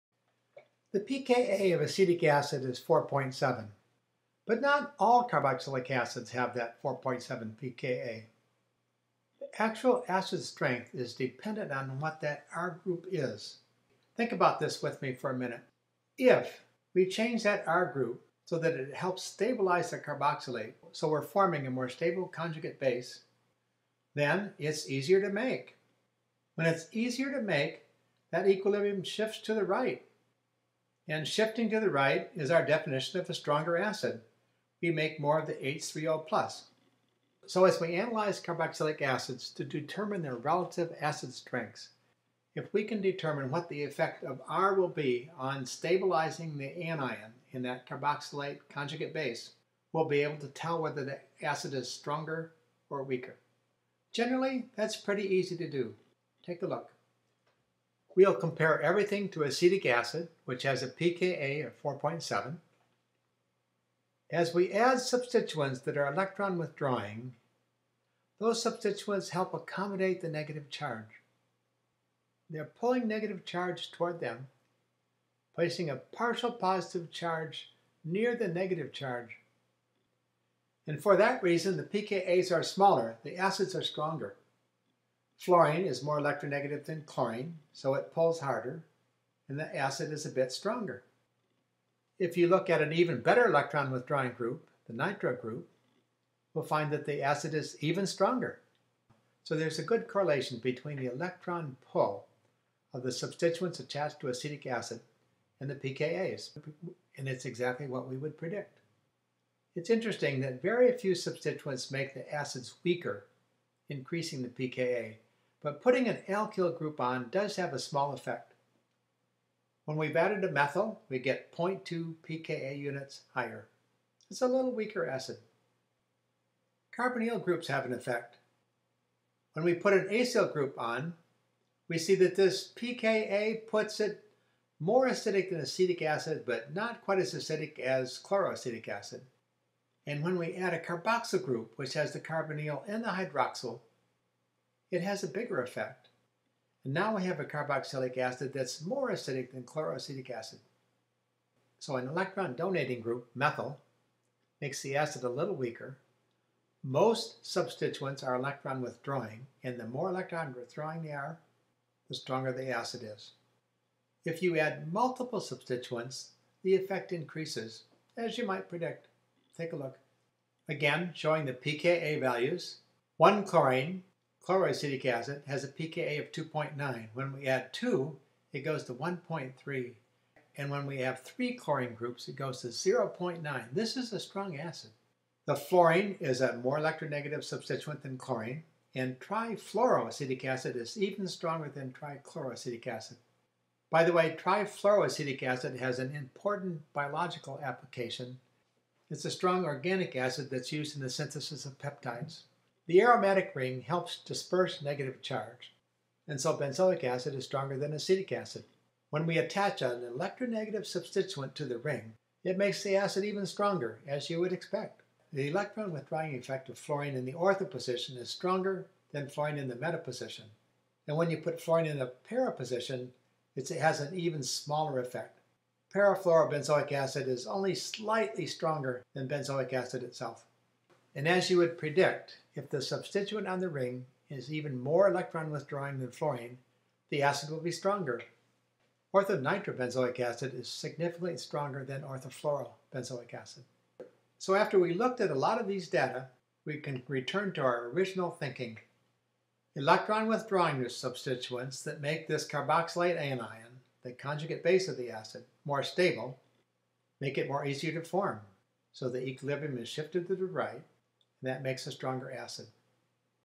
Strengths of Carboxylic Acids
Describes the common differences in the strengths (pKa values) of carboxylic acids and provides the common rationale for understanding those differences.